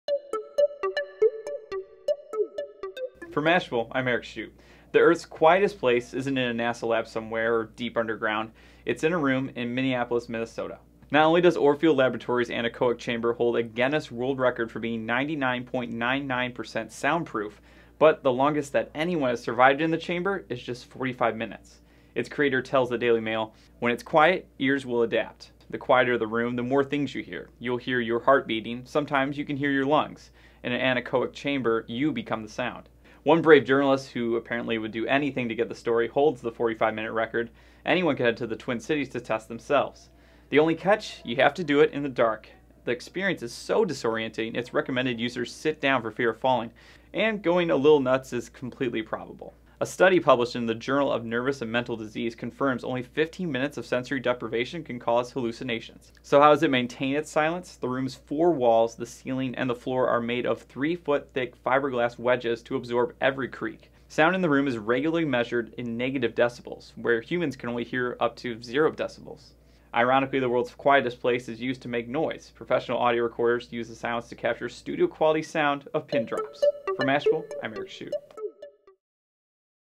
In the World's Quietest Room, You can Hear Your Heartbeat | Mashable
Orfield Laboratories' 'anechoic chamber' holds a Guinness World Record for being 99.99% sound proof. The room is in Minneapolis, and the longest anyone has lasted inside is 45 minutes.